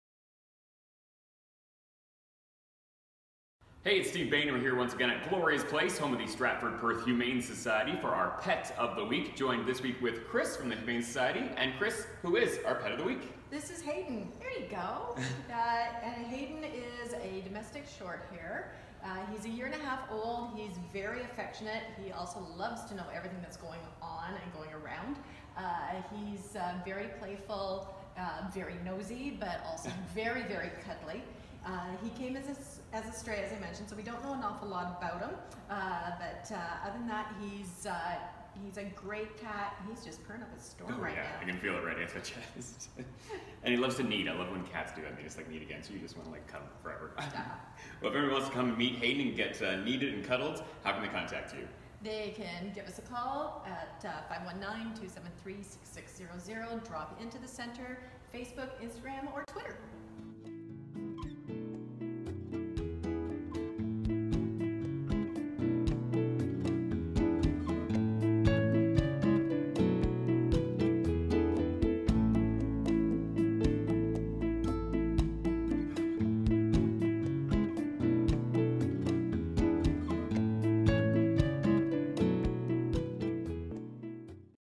Pet of the Week - Hayden
Every week we showcase an animal up for adoption at the Stratford-Perth Humane Society and today we want you to meet Hayden! He's a year-and-a-half old stray cat, who LOVES cuddling! You can see just how cuddly he is by checking out our video below, then be sure to head to see ALL the animals currently looking for their FUR-ever homes!